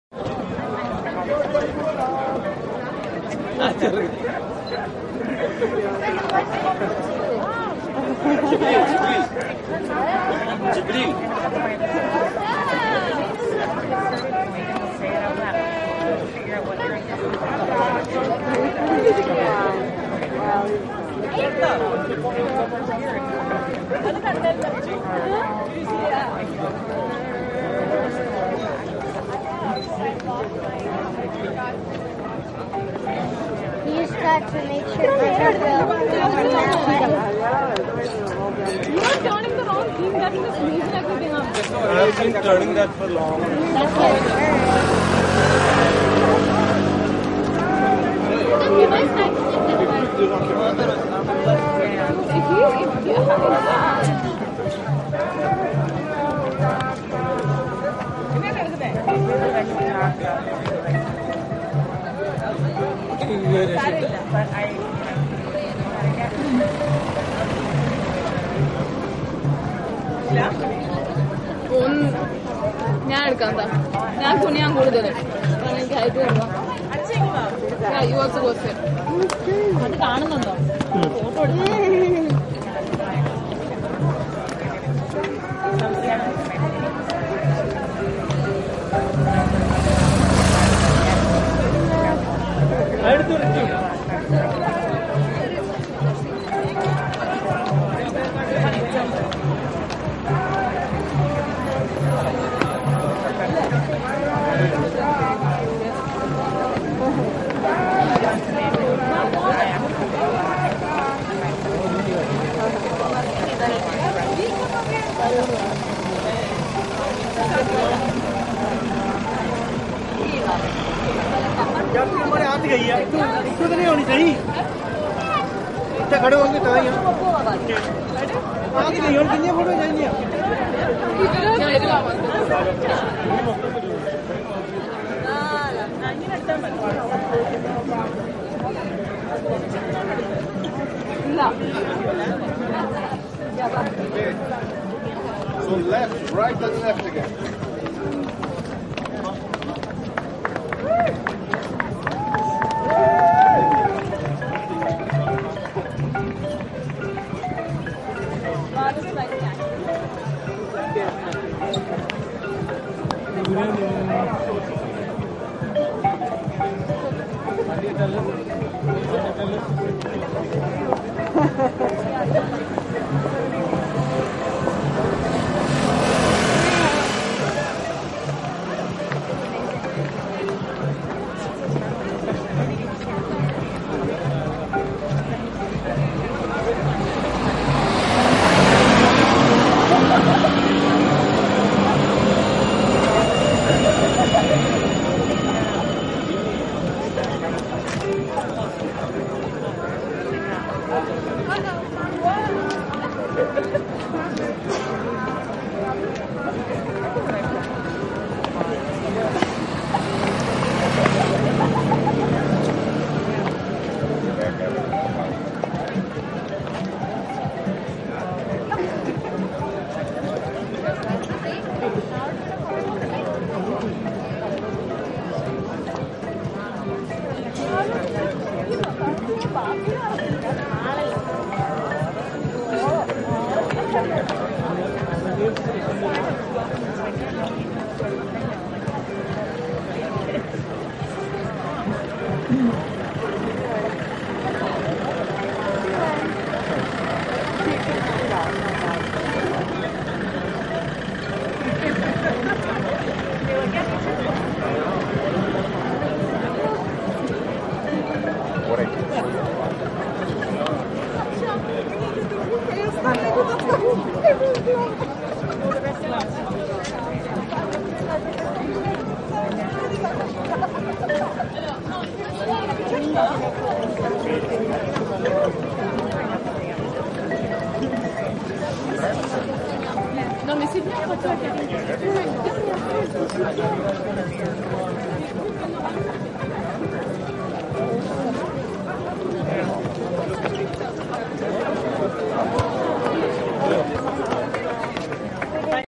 Freestyler Soccer Juggling at Sacre Coeur
On the last day of our visit to Paris, we decided to take in the view from the steps of Sacre Coeur, in the 18th arrondissement.  A big crowd of sightseers were on hand to watch the sun dip low in the horizon, but to our surprise we were also treated to the freestyling antics of Iya Traore.  Here's a 5 minute unedited clip that I took of him  while we chilled on the steps... and yes, we tipped him for his performance.